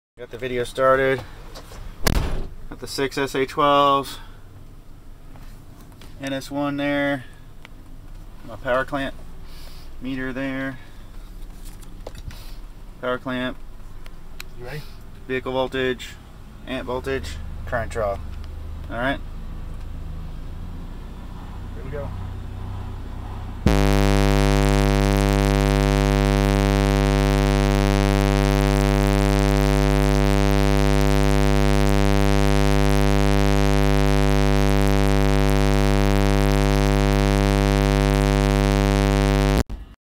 NS-1 Testing on 6 SA-12s
12V testing in Derricks explorer on 6 SA-12s